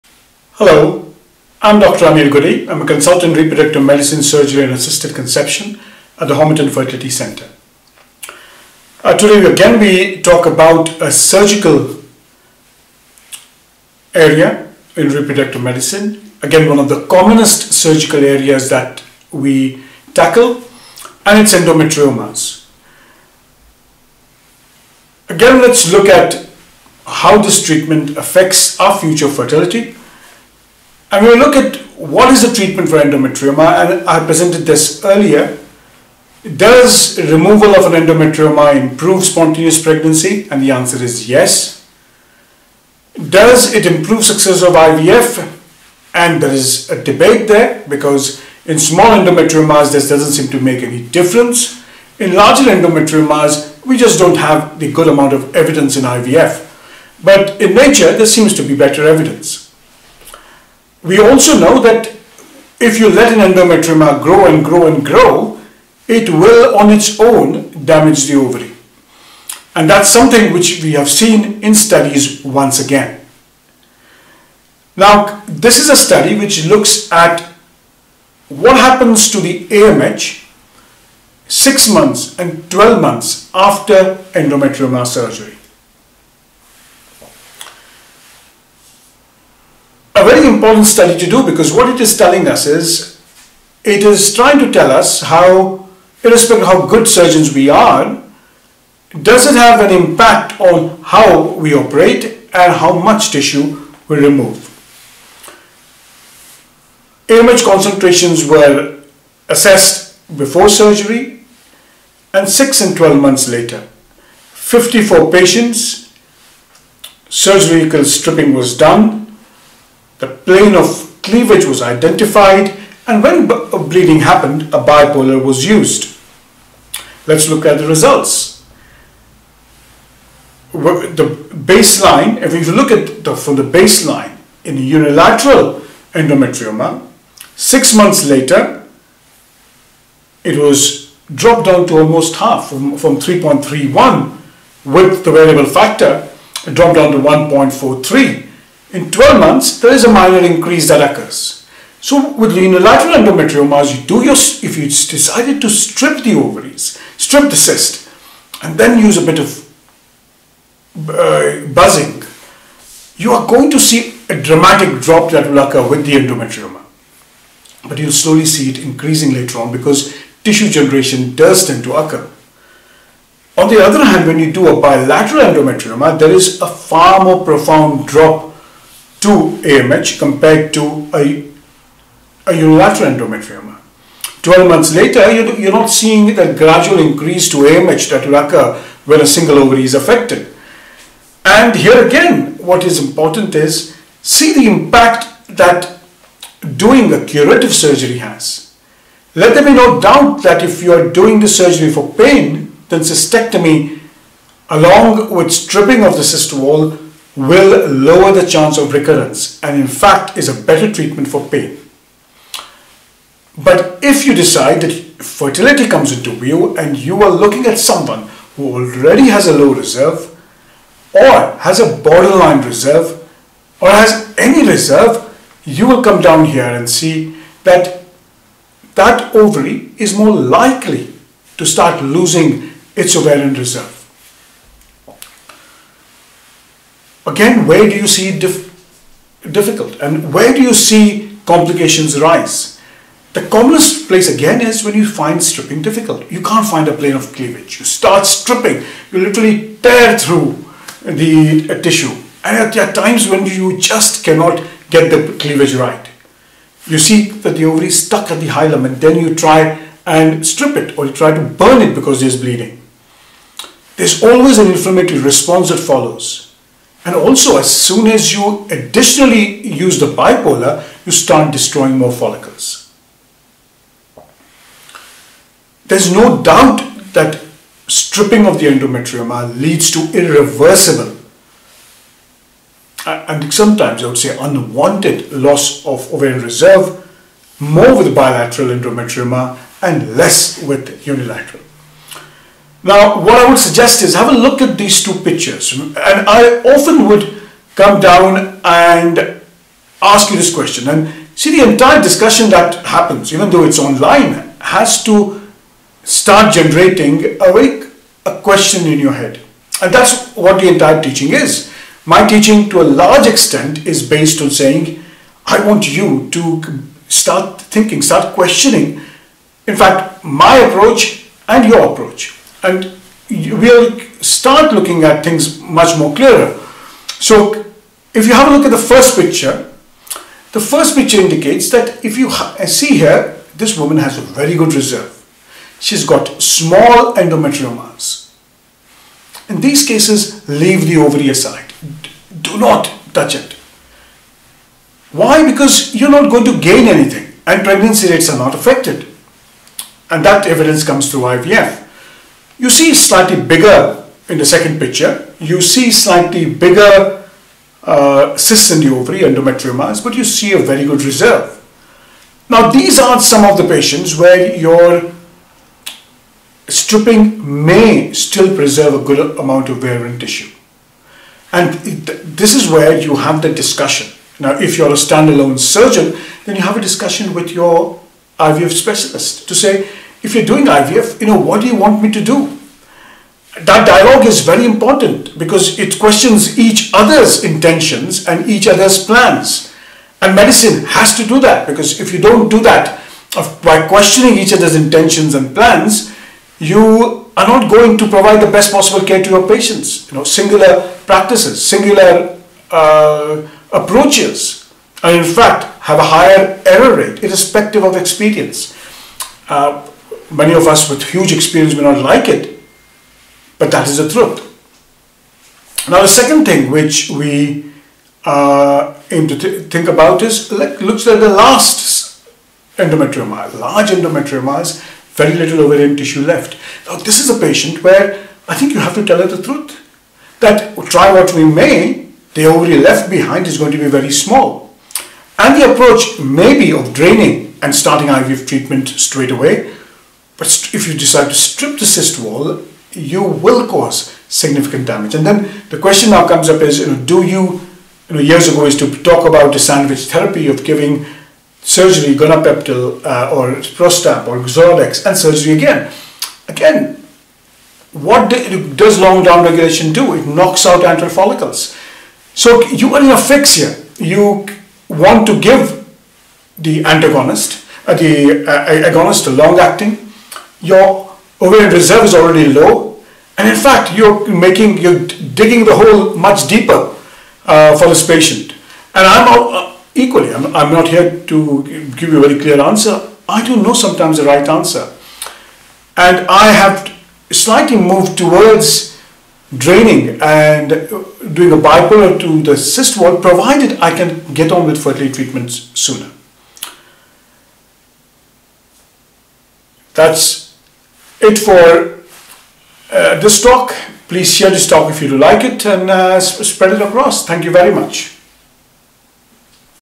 Endometrioma treatment and decline of AMH
Endometrioma (Endometriotic cysts) are one of the commonest benign cysts in the ovary. Surgery is an option to relieve pain and prevent recurrence. Does the AMH (Ovarian reserve )decline after treating the endometrioma and how much does it decline